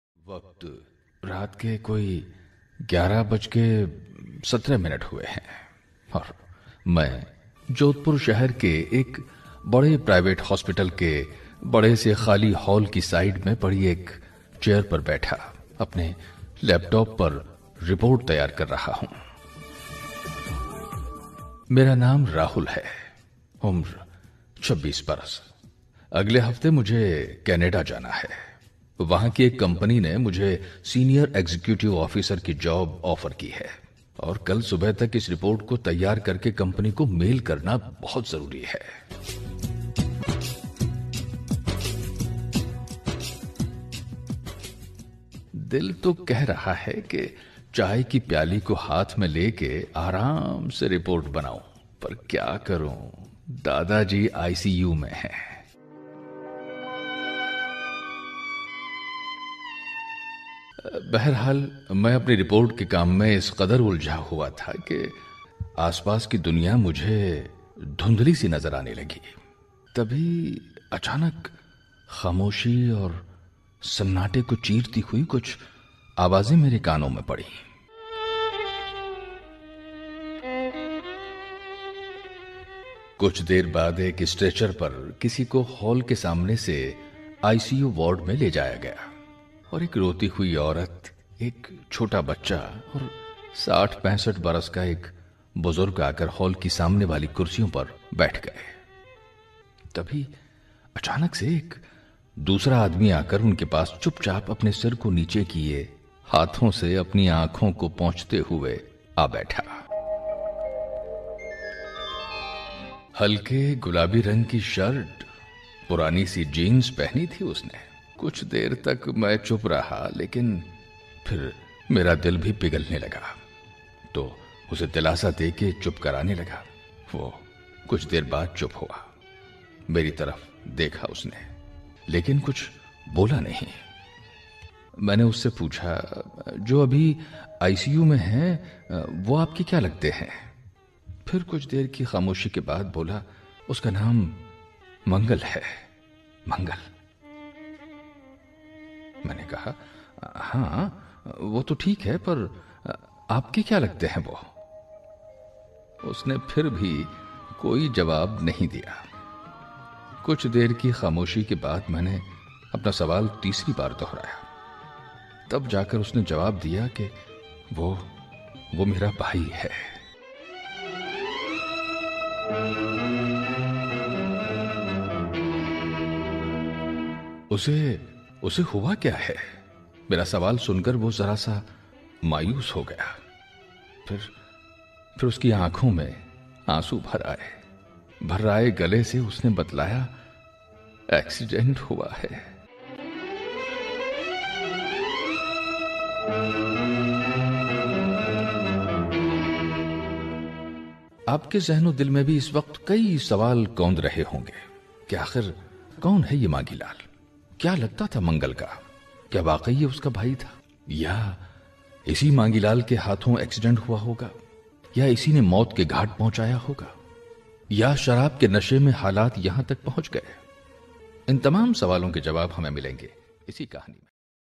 Ehsaas (Promo) || Story || Zafar Khan Sindhi || Dileep Kessani
Ehsaas (Story) - Releasing Soon

Voice: Zafar Khan Sindhi
Story: Dileep Kessani